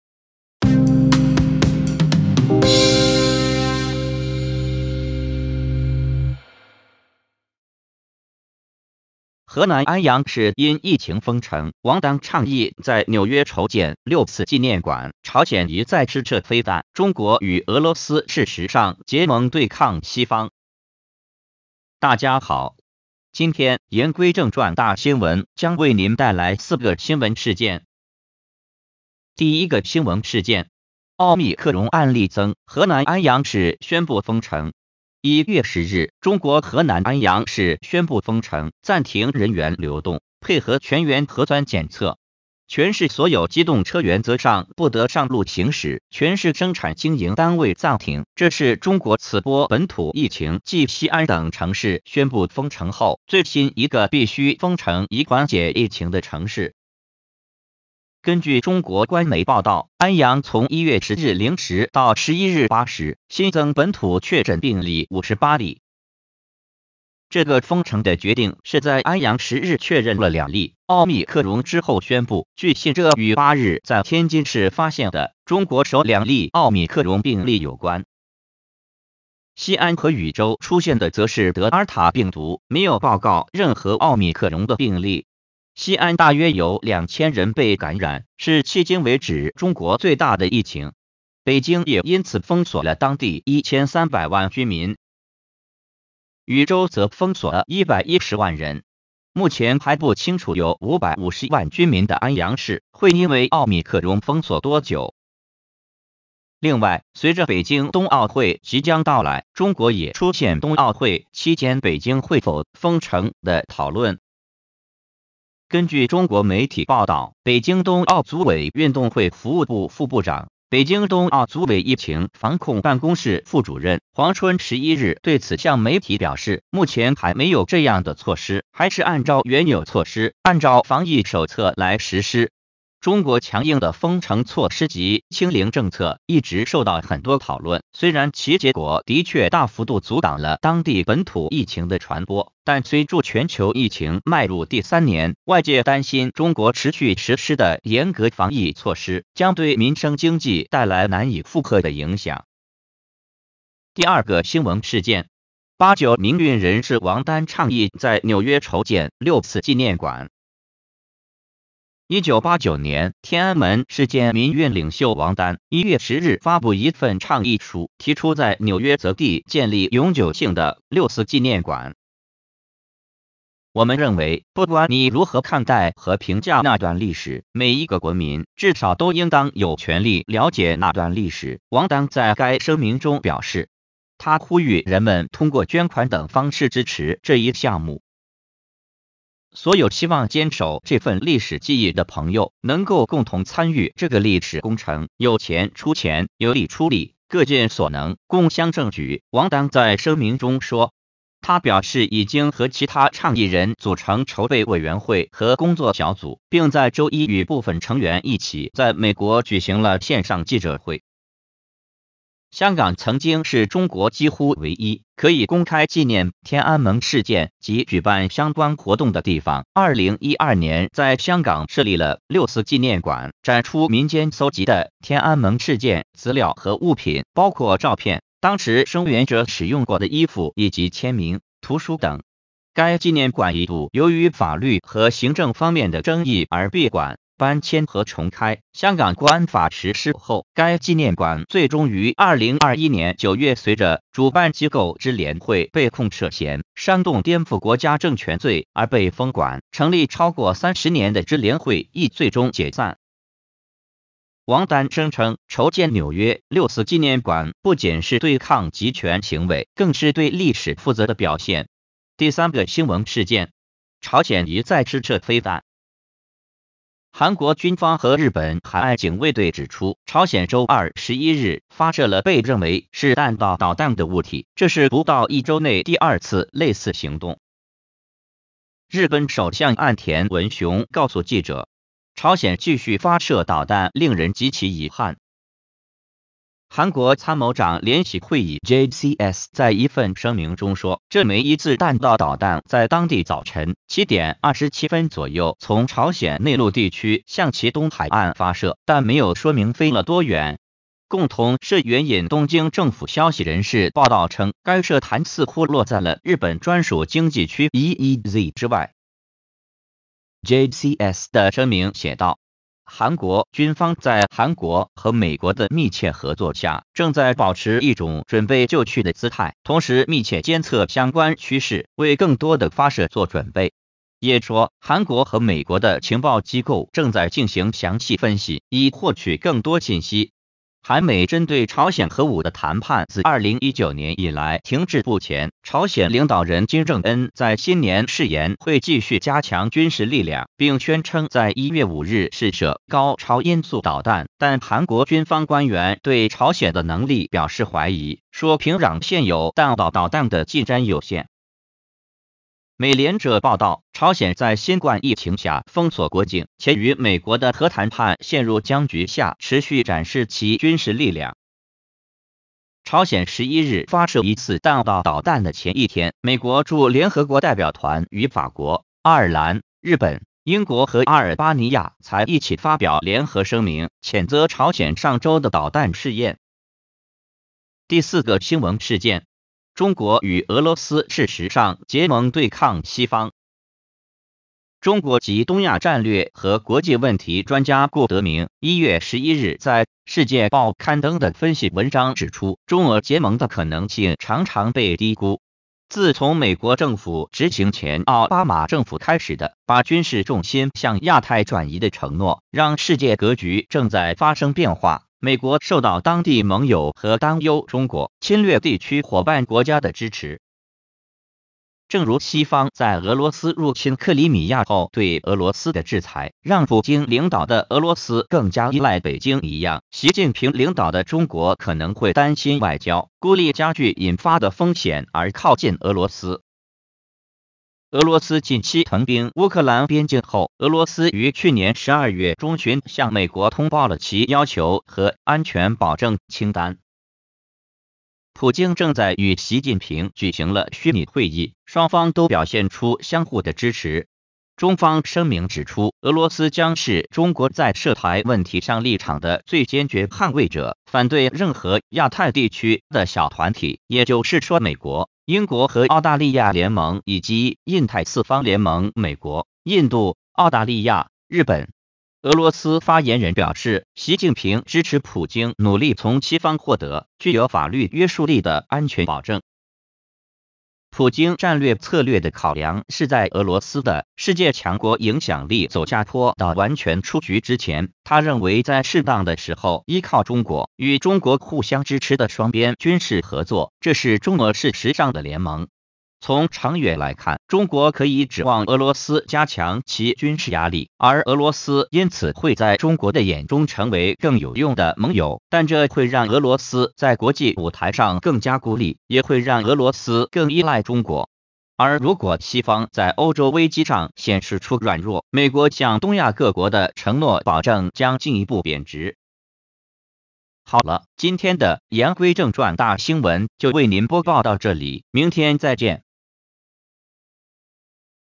河南安阳市因疫情封城 王丹倡议在纽约筹建“六四纪念馆” 朝鲜疑再试射飞弹 中国与俄罗斯事实上结盟对抗西方
00:00 2022年1月12日大新聞
00:29 河南安陽市因疫情封城
02:33 王丹倡議在紐約籌建「六四紀念館」
04:21 朝鮮疑再試射飛彈
06:17 中國與俄羅斯事實上結盟對抗西方